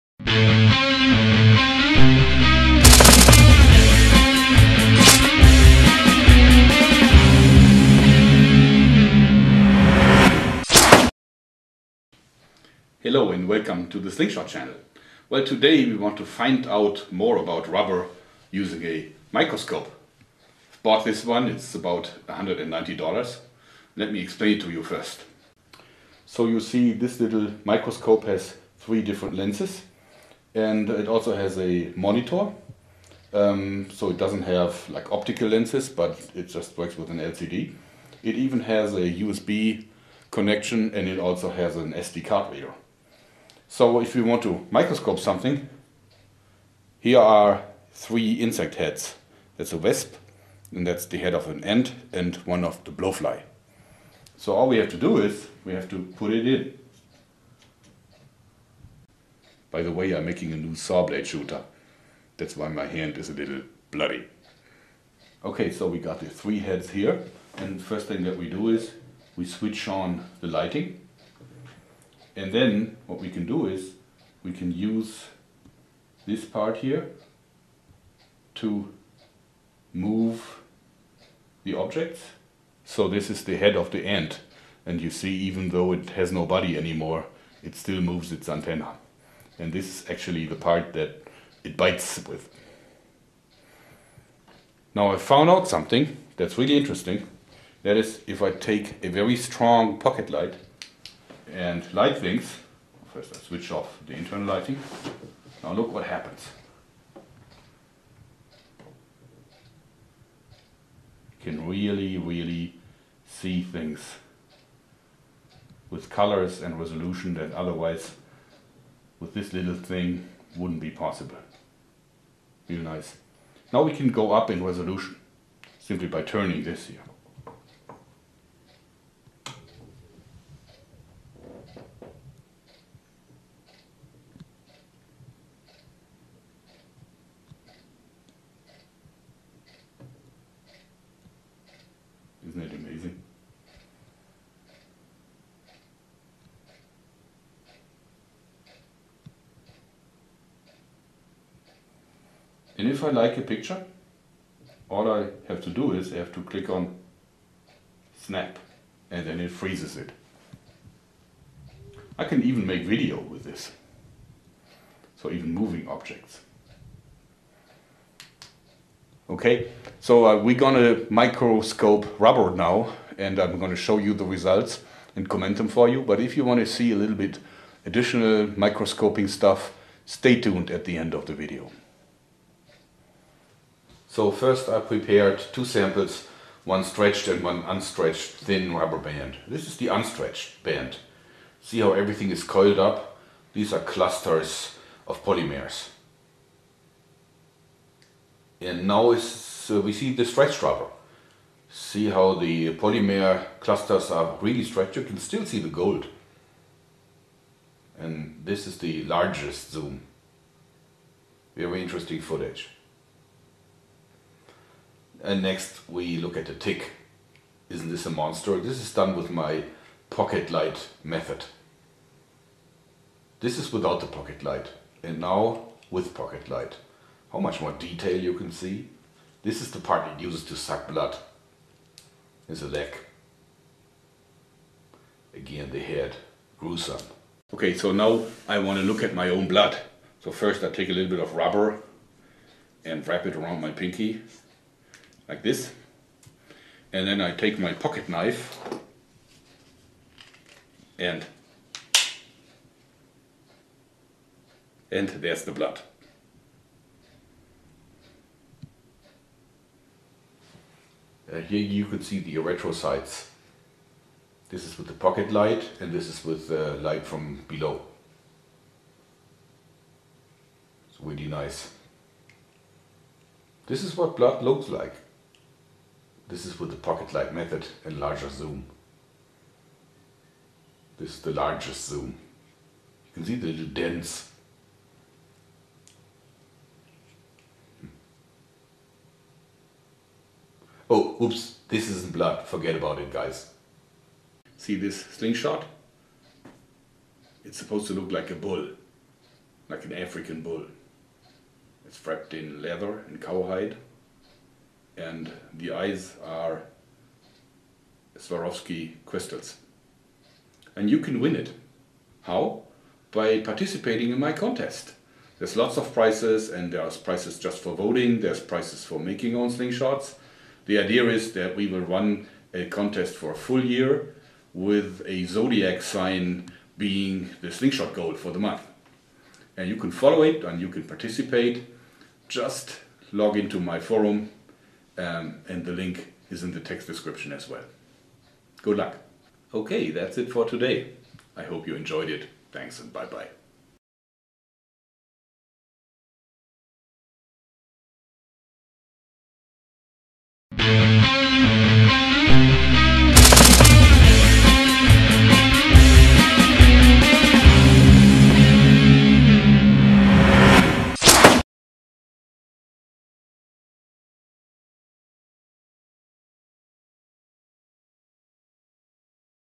Thera Band Gold Microscoped! PLUS Mega Slingshot Raffle
If you want to participate in the contest (prizes galore!), visit

Good luck!

In order to find out more about how rubber works, The Slingshot Channel uses a microscope to look at Thera band Gold in stretched and relaxed condition. The images are astounding!

In addition, insects and blood are microscoped, too.